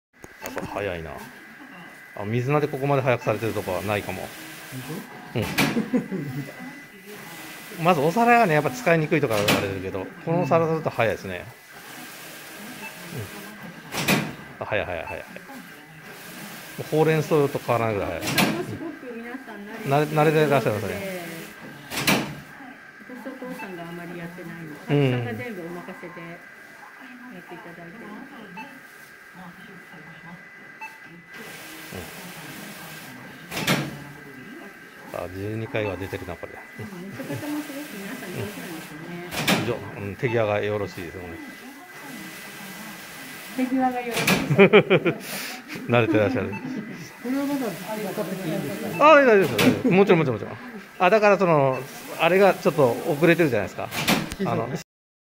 水菜の計量シーン（テーブルコンビα）
計量作業の省力化をご検討している皆様の参考動画です。
計量物：水菜
計量目標値：200～220g
計量方法：組合せ計量（高速モード）
【ポイント】
計量皿に￥100ショップで購入されたランチョンマットを装着することで、長くてかさばる大きな水菜が計量皿にスムーズに入り、計量速度が上がっています。

㈱オーケープランニング
サクッと計量！サクッと帰宅！
㈱オーケープランニングは計量を効率化するハカリを開発製造しています。